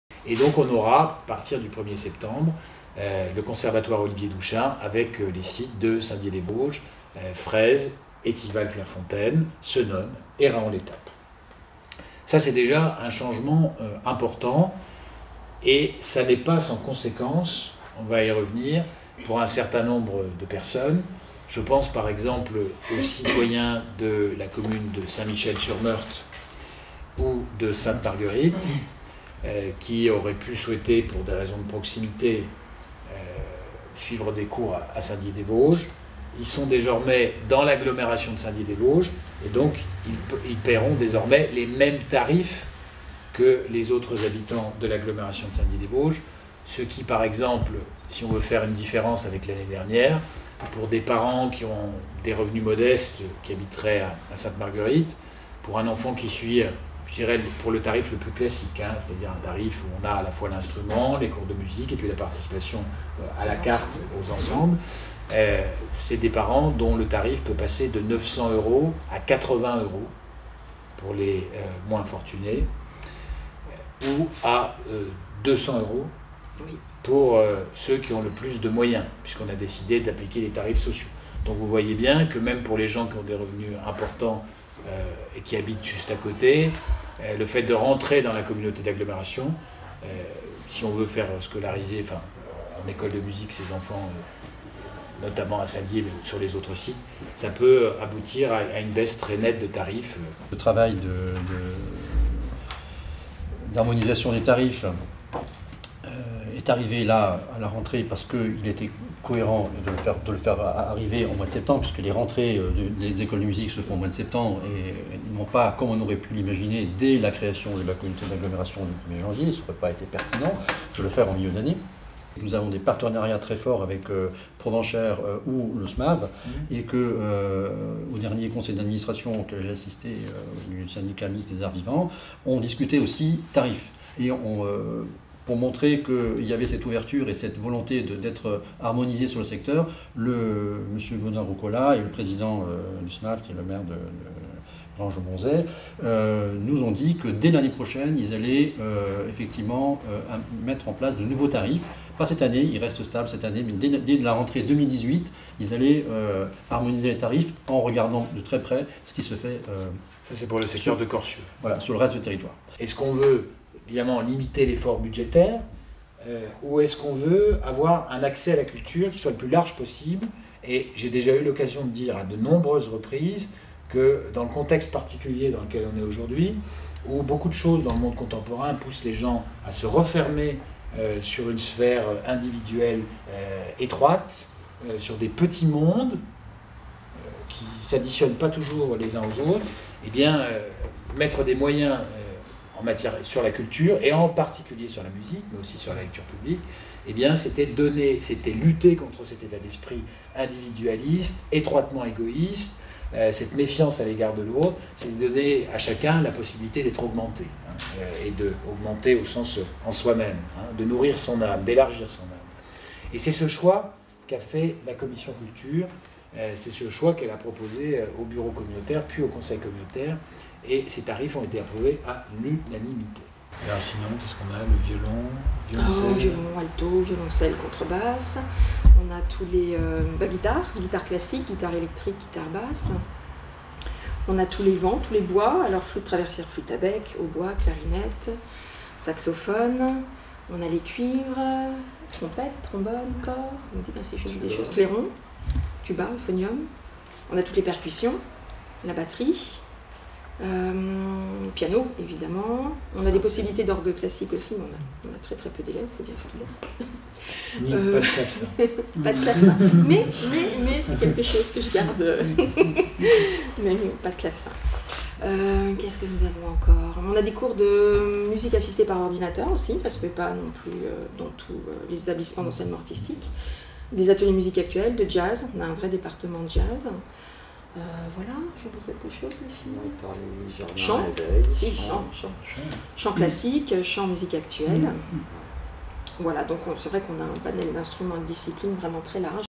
Une belle année musicale avec le conservatoire
Bonne nouvelle pour les habitants de la Communauté d’Agglomération qui souhaitent suivre un enseignement musical : ils pourront s’inscrire et payer le même tarif sur les cinq sites du Conservatoire Olivier-Douchain : Fraize, Senones, Etival-Clairefontaine, Raon L’Etape ou le site central de Saint-Dié-des-Vosges.
Une économie substantielle, les tarifs pouvant ainsi passer de 900 € (le coût réel d’un enseignement pour un élève) à 80 € (pour les foyers avec des revenus modestes) !
800 élèves étaient inscrits la saison passée, sur les 5 sites soit 1 % de la population de l’Agglomération.

Les cours reprendront le 18 septembre 2017.